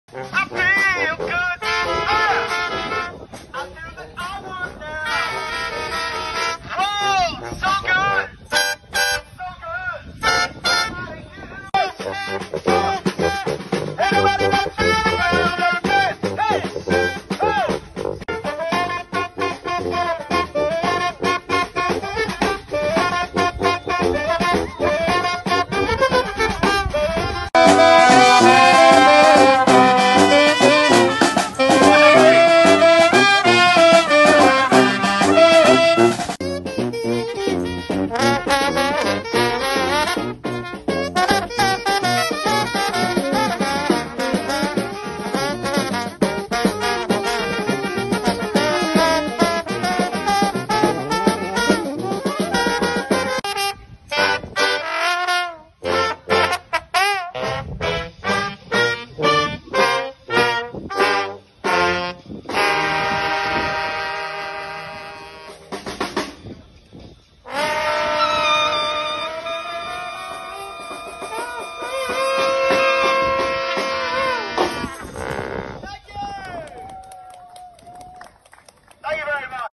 Bigtime Brass - Live Street Performance - AliveNetwork.com
Delivering an explosive fusion of funk, soul and pop, Bigtime Brass Band are fun loving, energetic and guarantee a good time! Performing completely acoustic & in a carnival style (PA available on request), get ready for a full celebratory atmosphere!